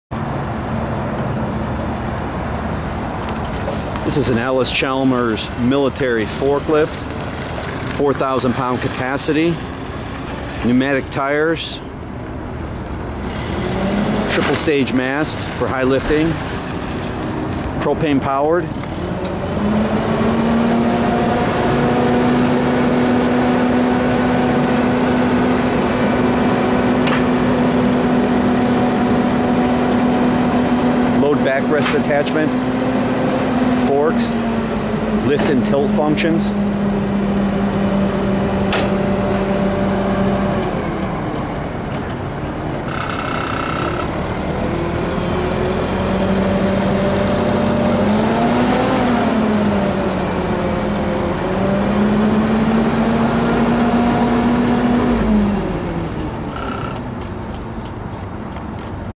Military Forklift - FP40 Allis Chalmers Vintage 4000lb
Allis Chalmers model FP40 military forklift, military olive green color, lpg powered, unit has a new muffler, new lpg regulator, some new hoses, good engine, lpg-propane powered, automatic transmission, 4000lb capacity, load back rest attachment, key starts and runs, no smoke, no known leaks, pneumatic tires, forks, reupholstered seat.